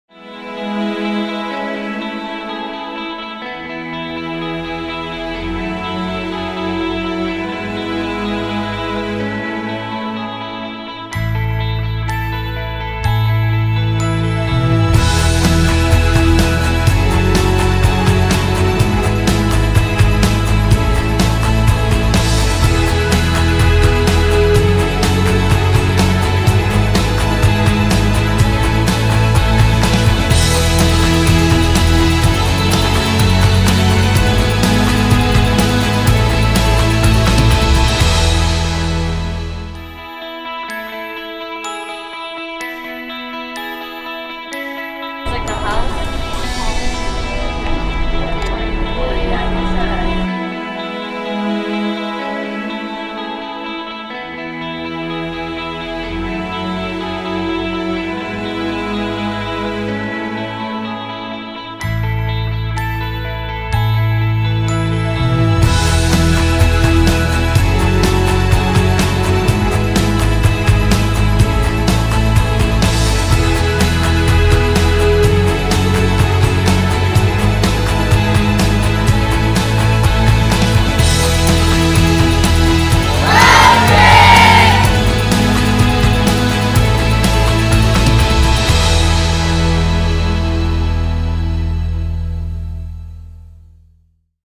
Road Trip 2015: Journey to Success
Road Trip is a Stanislaus County office of Education program designed to help students adjust to life in a jr. high setting